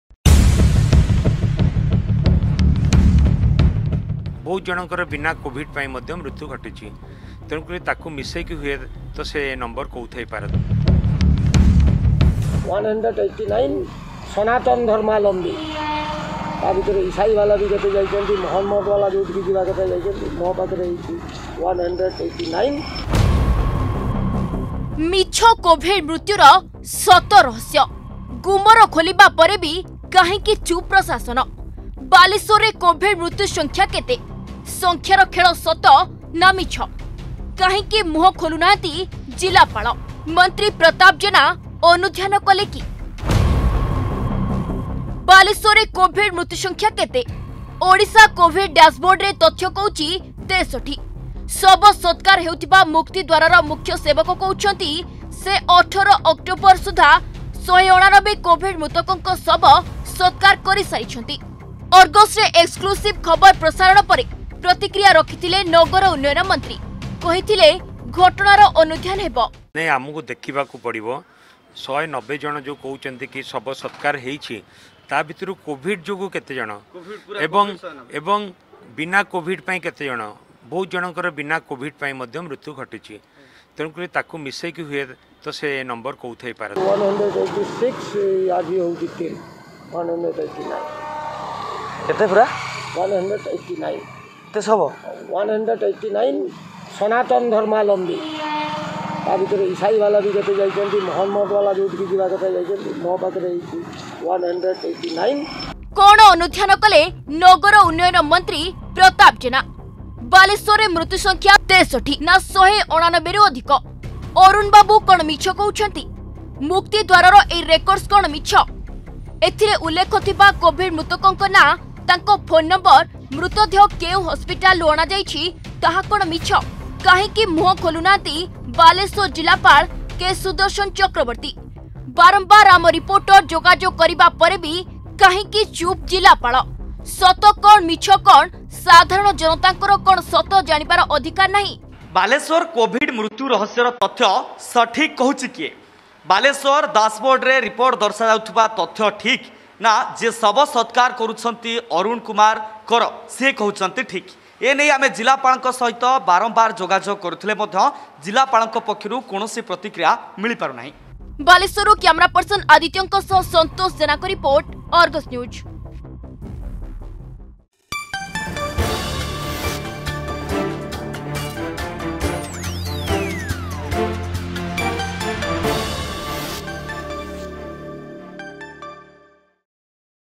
ମିଛ କୋଭିଡ୍ ମୃତ୍ୟୁର ସତ ରହସ୍ୟ
ବାଲେଶ୍ବରରେ କୋଭିଡ୍ ମୃତ୍ୟୁ ସଂଖ୍ୟା କେତେ । ଓଡ଼ିଶା କୋଭିଡ୍ ଡ୍ୟାସବୋର୍ଡର ତଥ୍ୟ କହୁଛି 63 । ଶବସତ୍କାର ହେଉଥିବା ମୁକ୍ତିଦ୍ବାରର ମୁଖ୍ୟ ସେବକ କହୁଛନ୍ତି ସେ 18 ଅକ୍‌ଟୋବର ସୁଦ୍ଧା 189 କୋଭିଡ୍ ମୃତକଙ୍କ ଶବ ସତ୍କାର କରିସାରିଛନ୍ତି .